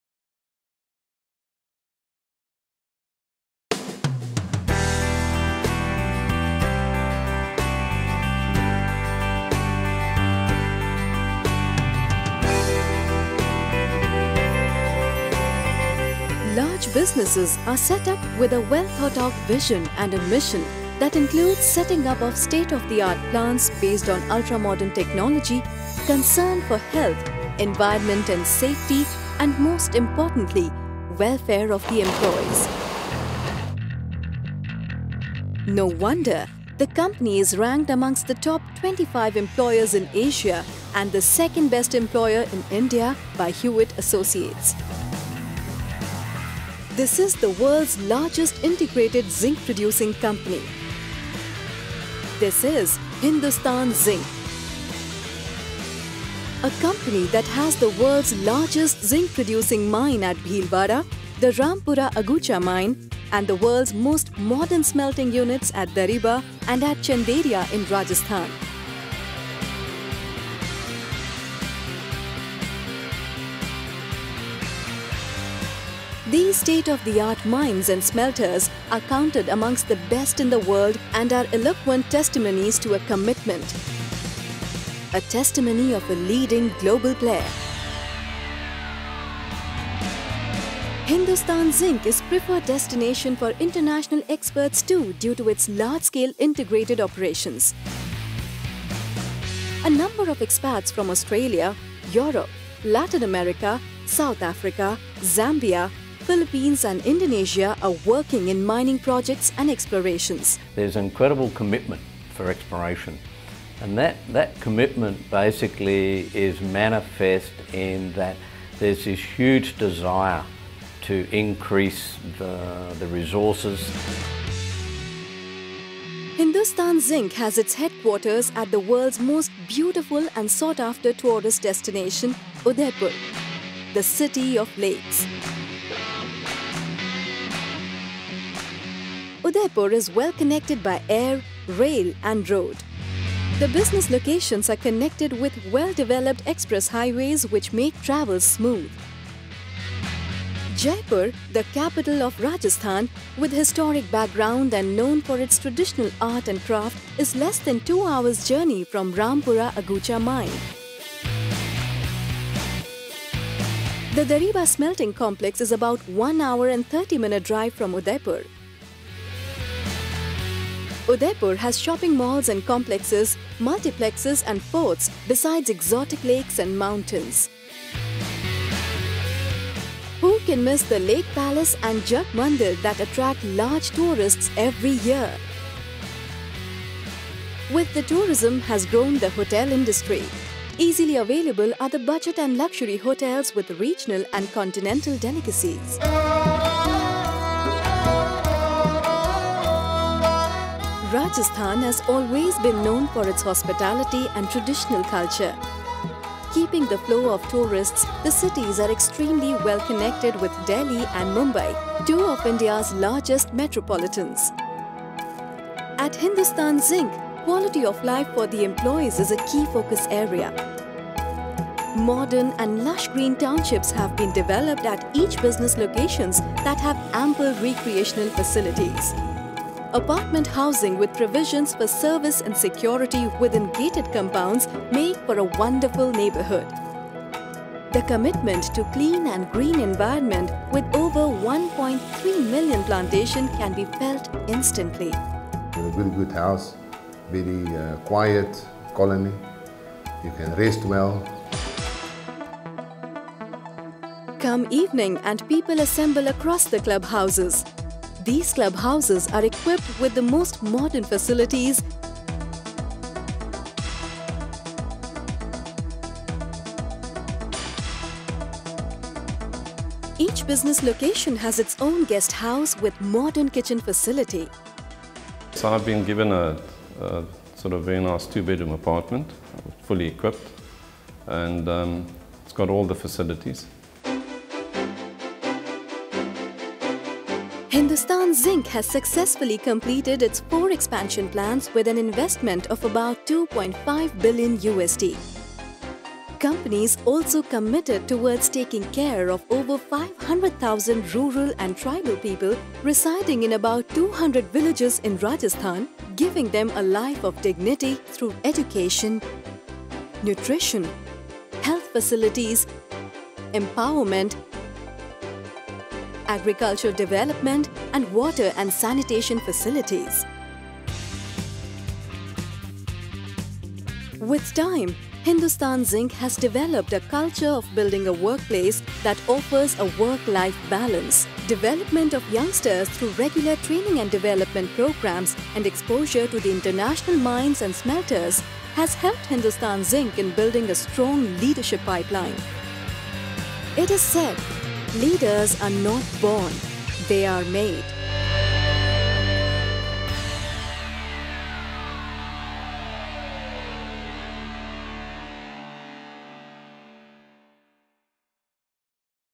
Hindustan Zinc - Human Resources(HR) Film
The quality of life at Hindustan Zinc, facilities available at the residential colonies, local culture and the key attractions of the nearby cities.

Hindustan Zinc - A Vedanta Group company in Zinc-Lead-Silver Business. The World's Largest Integrated Zinc Producer.